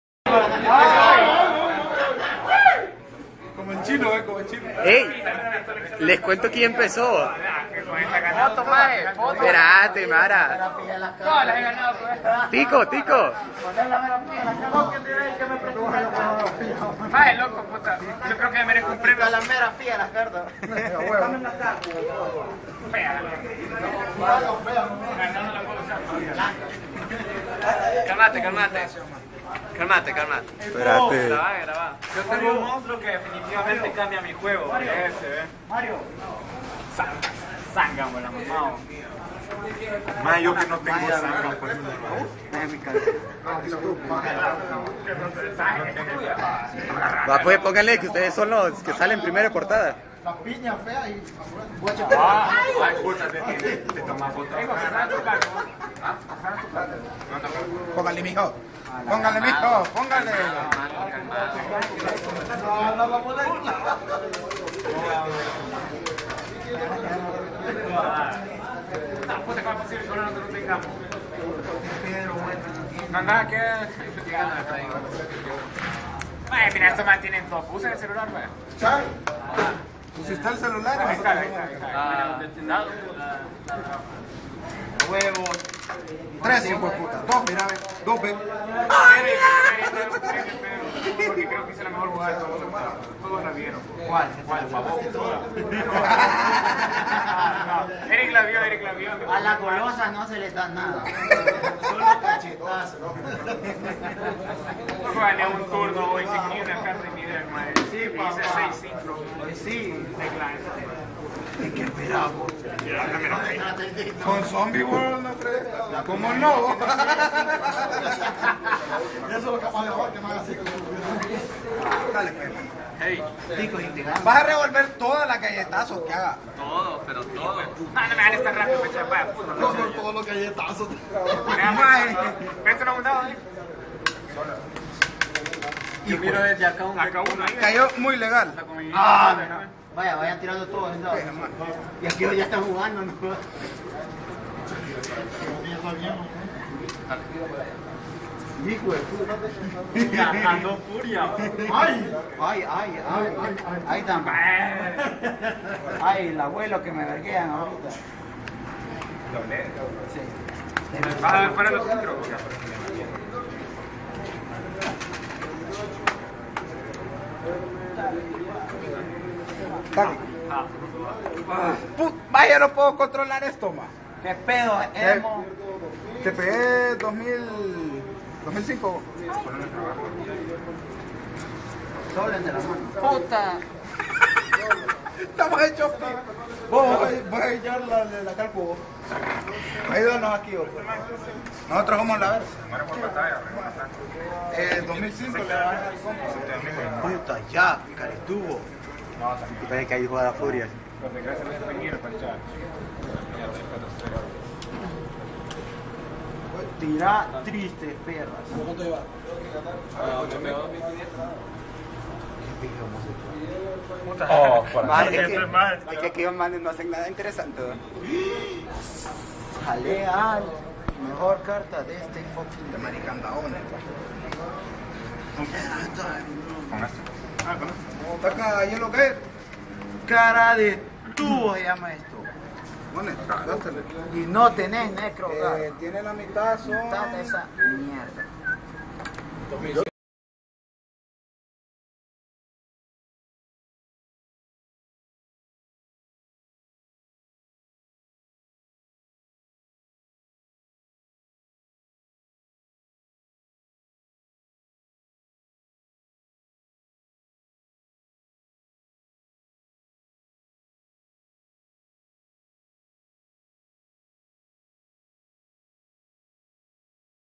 A look into the past (11 years): Yu-Gi-Oh! HN
Hace 11 años. Top8 en Little Caesar's. no fights. just yugi, food and friends